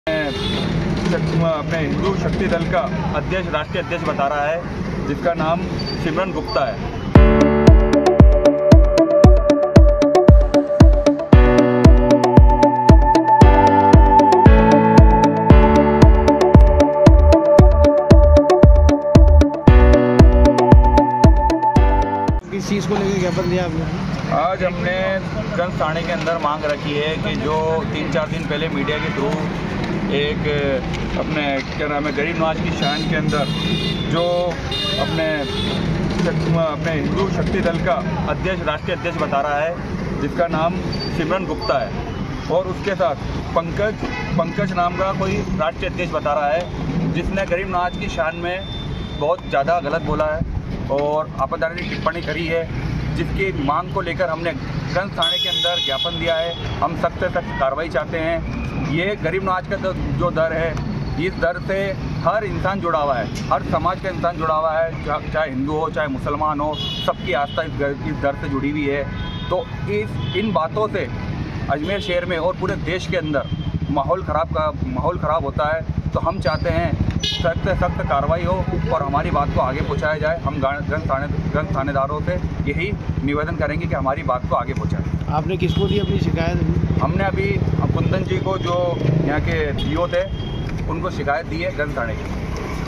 Ajmer |लौंगिया क्षेत्रवासियों ने गंज थाना प्रभारी को ज्ञापन देकर सौहार्द और धार्मिक | AYN NEWS
लौंगिया क्षेत्रवासियों ने  गंज थाना प्रभारी को ज्ञापन देकर सौहार्द और धार्मिक भावना आहत करने वालों के खिलाफ कार्रवाई की मांग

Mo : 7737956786